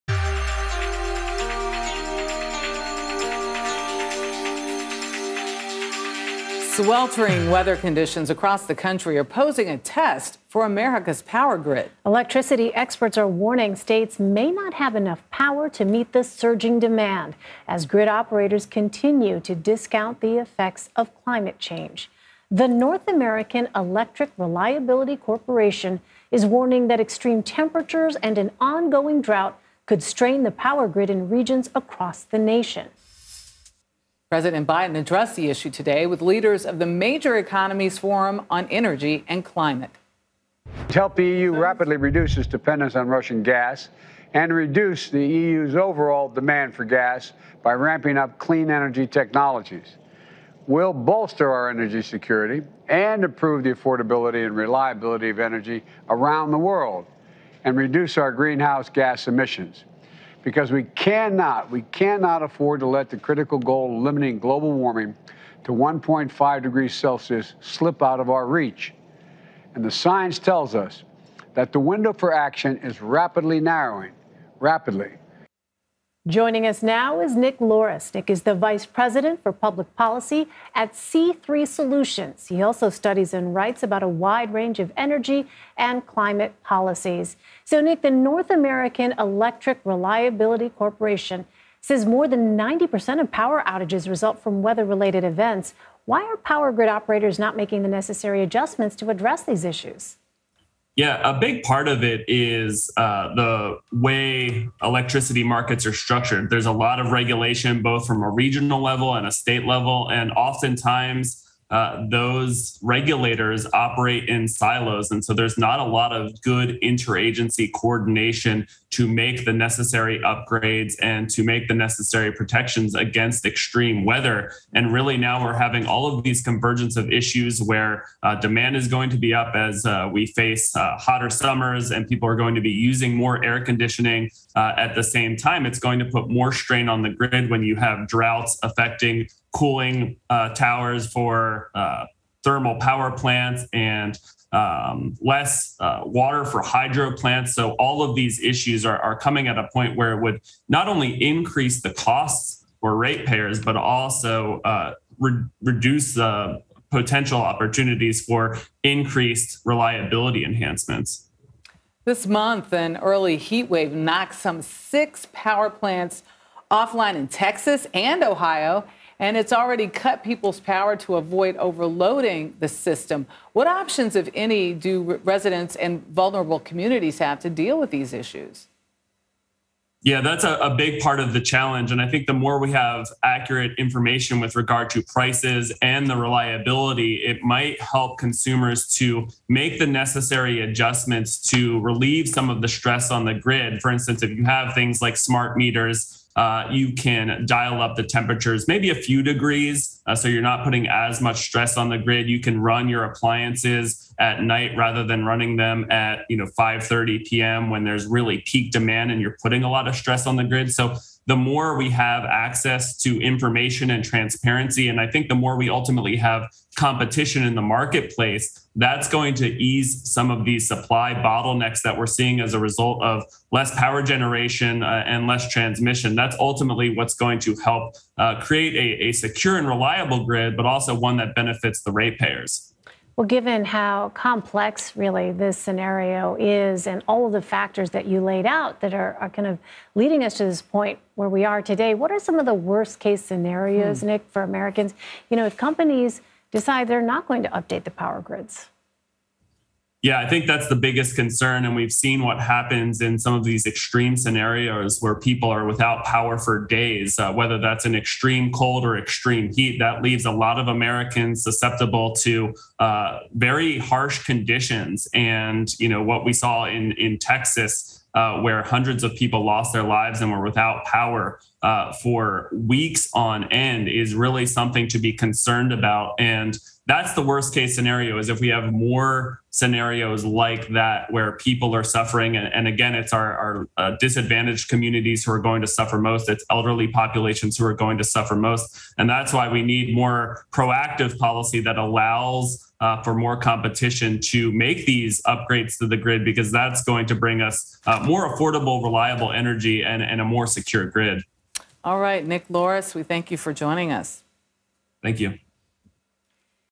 Climate change tests U.S. electricity infrastructure
Heat waves across the country are posing challenges for America's power grid, and experts warn that states may not have enough electricity to meet surging demand as grid operators continue to discount the effects of climate change. Nick Loris, the vice president for public policy at C3 Solutions, discussed the ongoing issue with CBS News anchors Michelle Miller and Elaine Quijano.